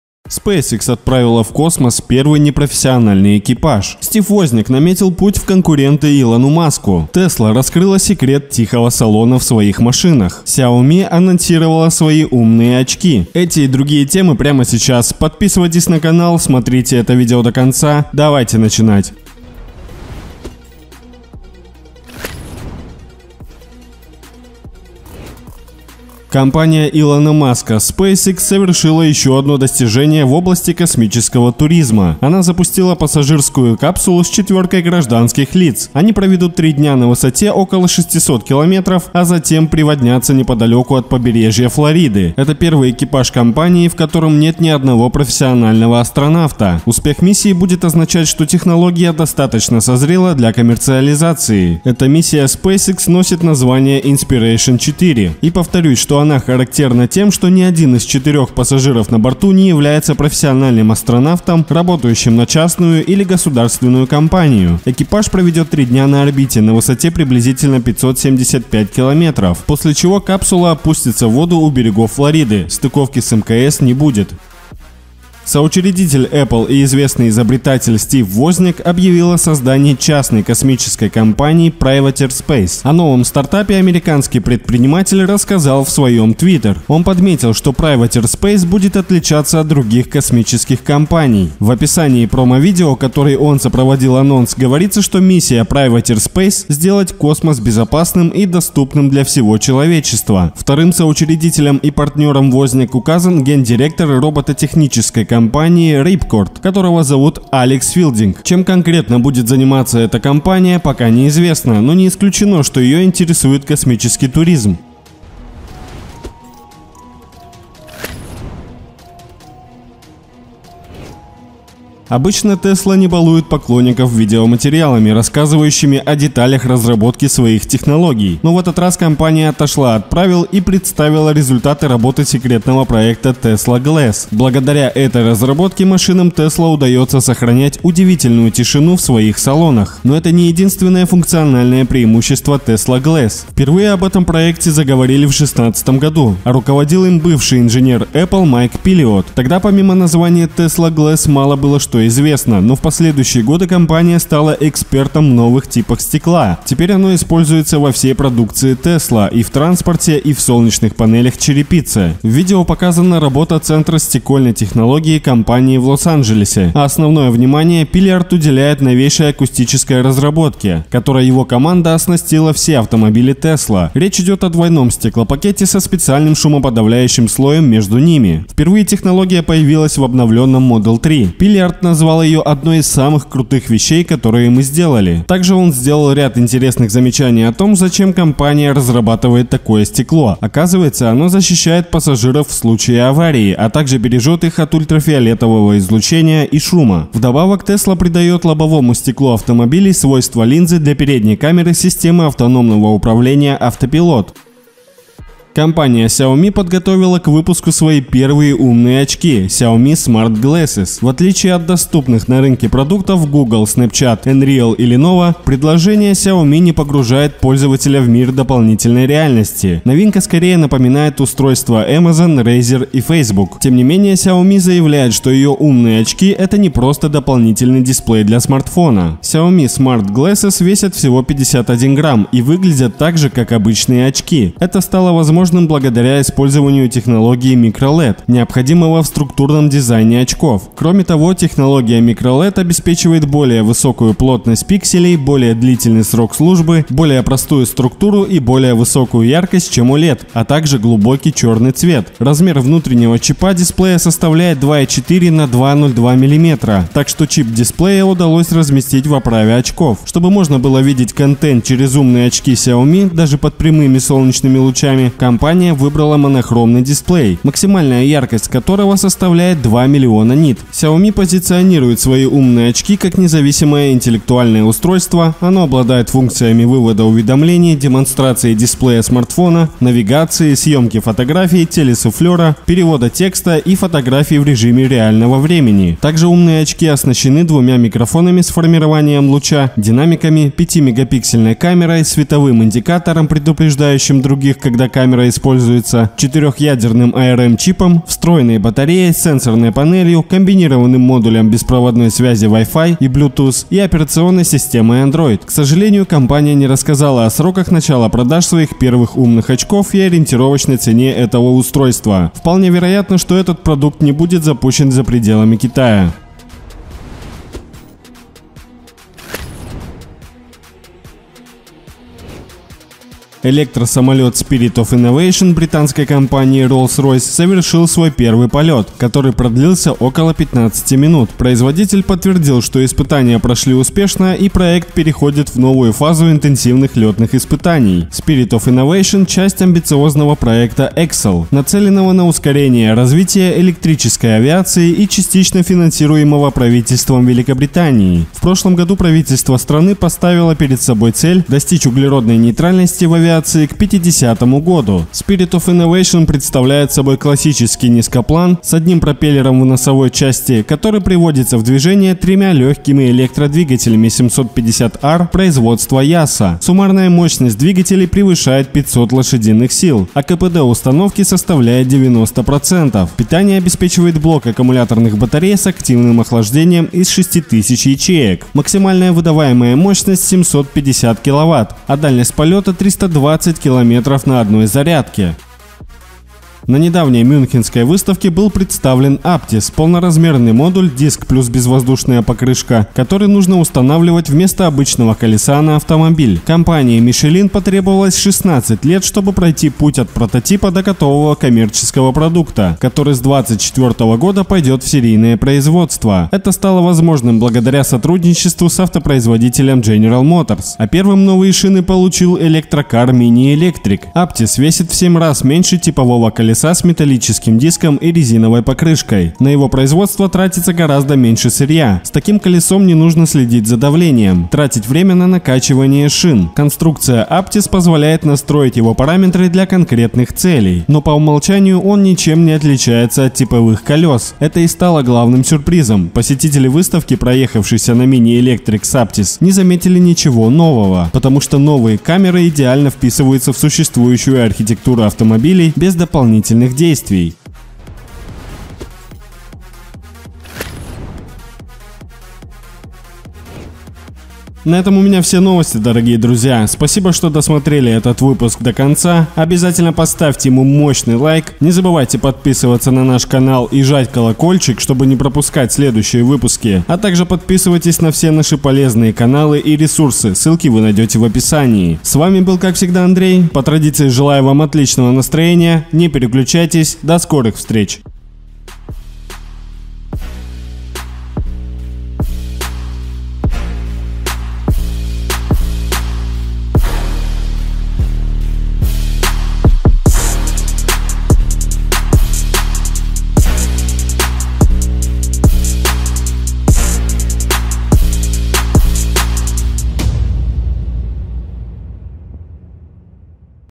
SPACEX INSPIRATION4 - ИЛОН МАСК СДЕЛАЛ ЭТО
👇 Отзывы из закрытого канала!

► В этом видео:
• SpaceX отправила в космос первый непрофессиональный экипаж. Стив Возняк объявил о создании Privateer Space — новой космической компании. Tesla раскрыла секрет тихого салона в своих машинах. Анонсированы умные очки Xiaomi Smart Glasses с функциями навигации, телесуфлёра, перевода и др. Электросамолет Rolls-Royce совершил первый полет. Безвоздушные шины Michelin готовы к серийному производству.

► Таймкоды:

00:00 - Коротко о главном
00:20 - SpaceX отправила гражданских в космос
01:14 - Стив Возняк создает новую космическую компанию
01:55 - Tesla раскрыла секрет тихого салона в своих машинах
03:24 - Умные очки Xiaomi Smart Glasses
05:17 - Электросамолет Rolls-Royce совершил первый полет
06:25 - Безвоздушные шины Michelin
07:28 - Подпишись на канал